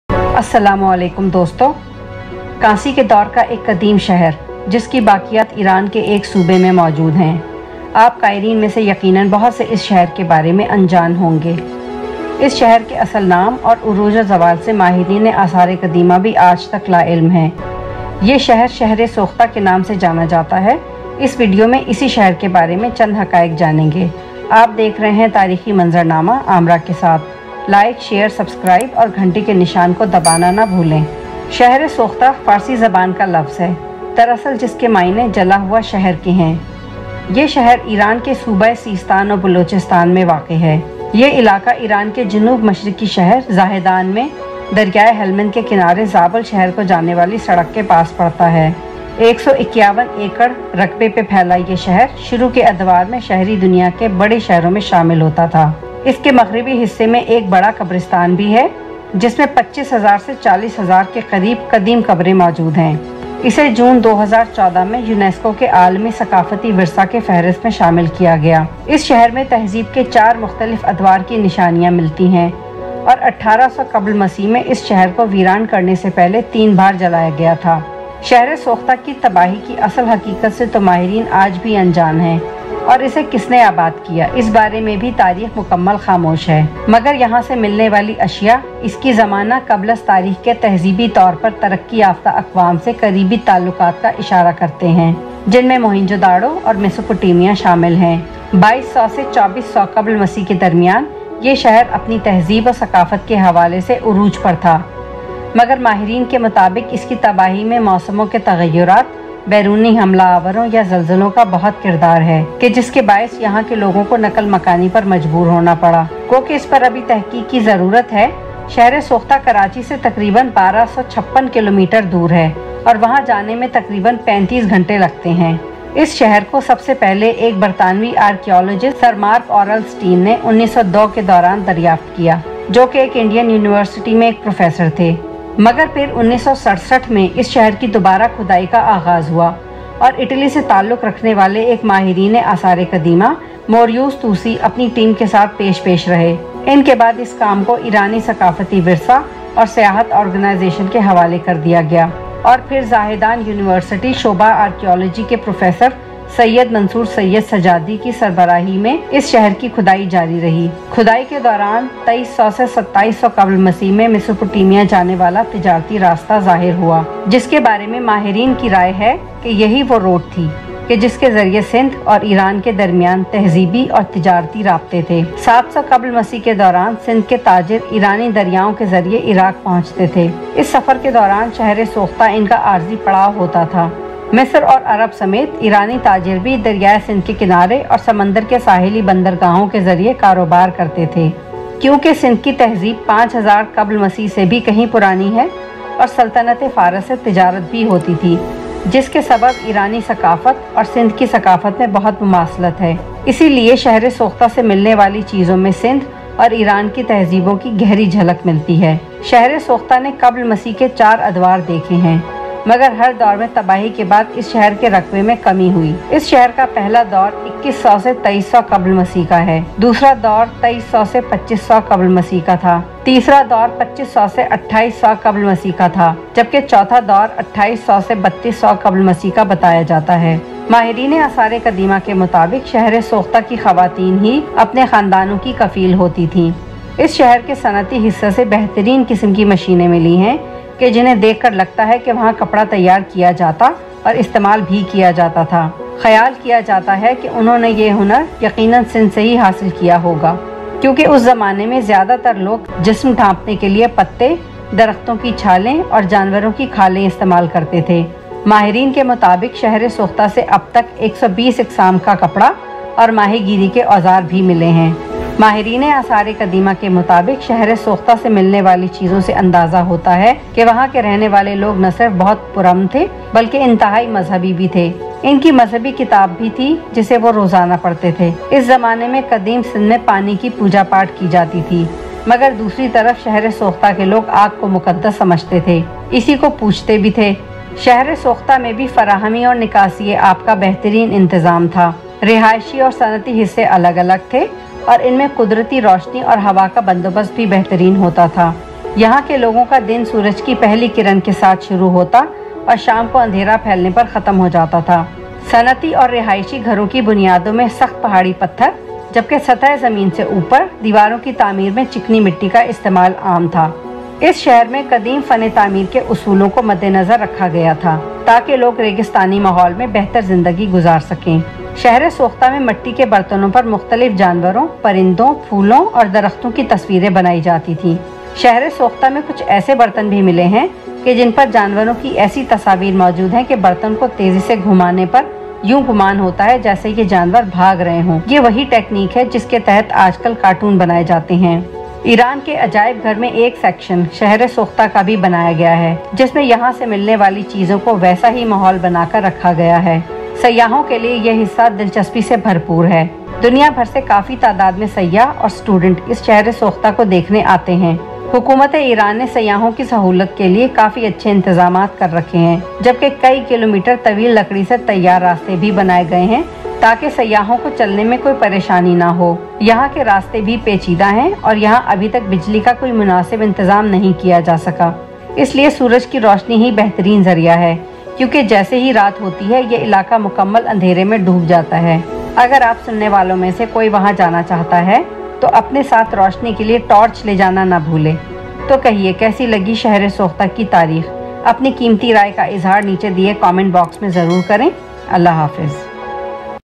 History of Shahr e Sukhteh || History with Amera || S1E12 || Documentary of Shahr e Sukhta in U/H.
Shahr e Sukhta meaning burn city is an archaeological site of Bronze Age.It is located in Sistan and Baluchistan Province of Iran on the bank of Helmand River near the Zahedan - Zabol road.It was placed on the UNESCO World Heritage List in June 2014

Shahr e Sukhta

burn city

archaeological sites

Bronze Age

Helmand River

ancient city

شہر سوختہ